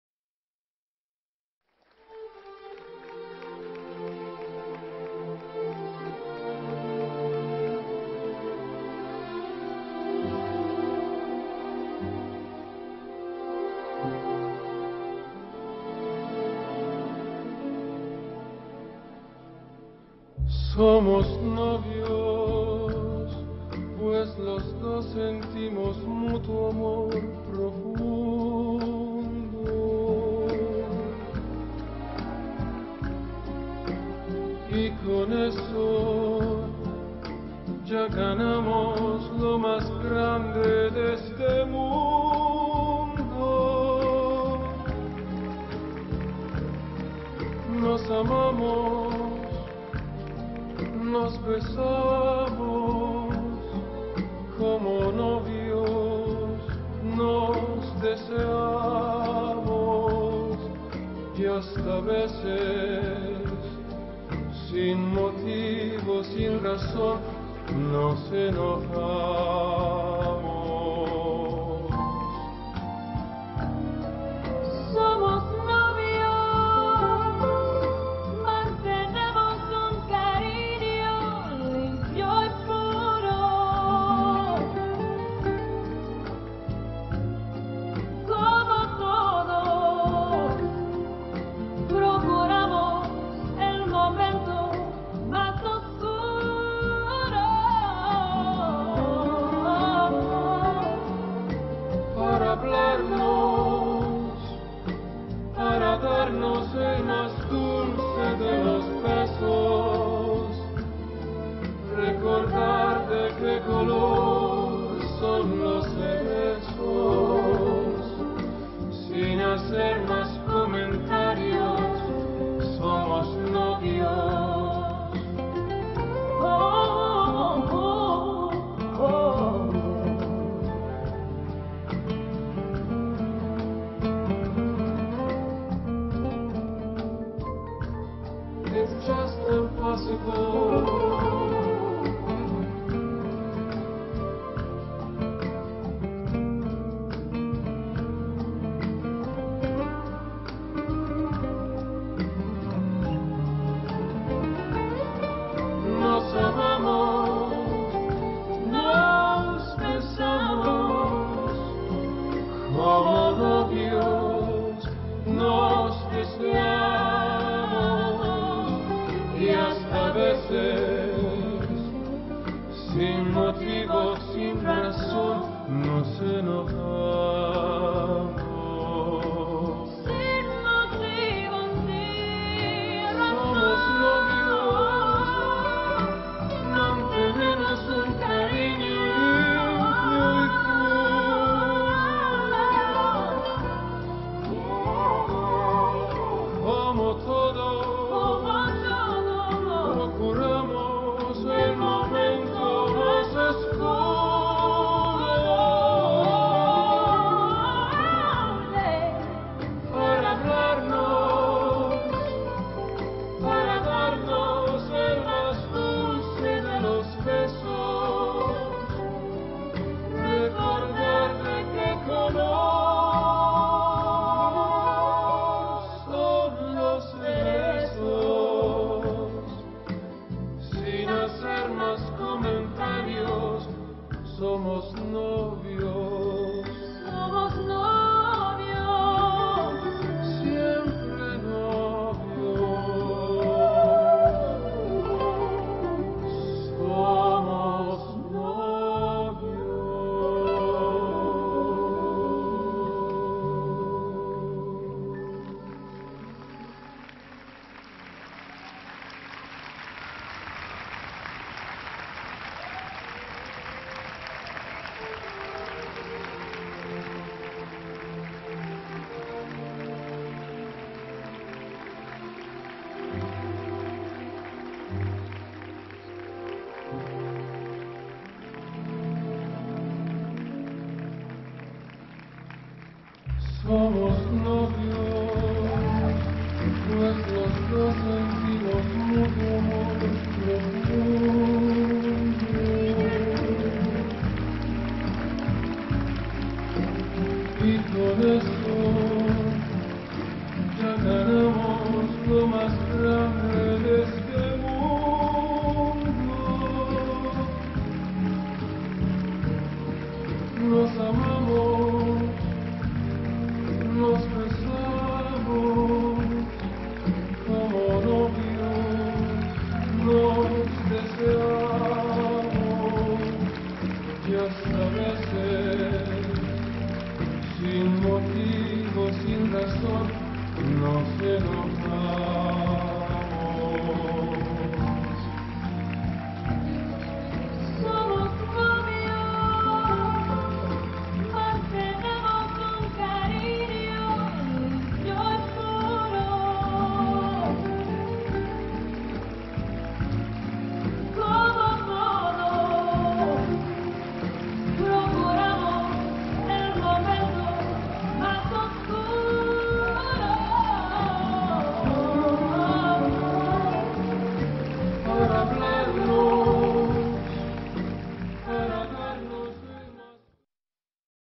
Slavik Kryklyvyy and Elena Khvorova WSS 06 Rumba
Rumba performed by Slavik and Elena at World Super Stars 2006